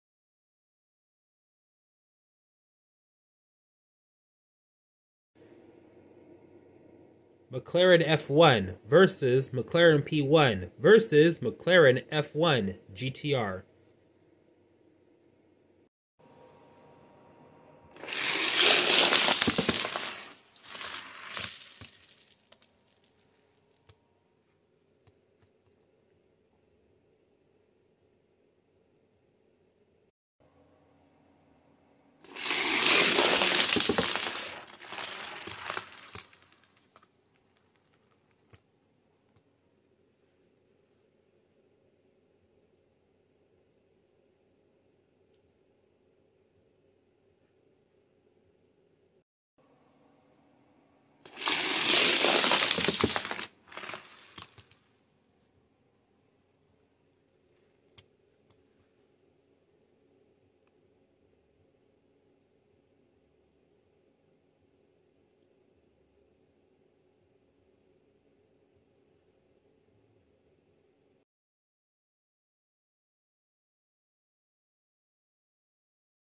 Hot Wheels Versus (3-Lane Raceway): McLaren F1 vs McLaren P1 vs McLaren F1 GTR
We got a Generational McLaren Race where we see the F1, Its GTR Variant & P1 All in today's Versus Race...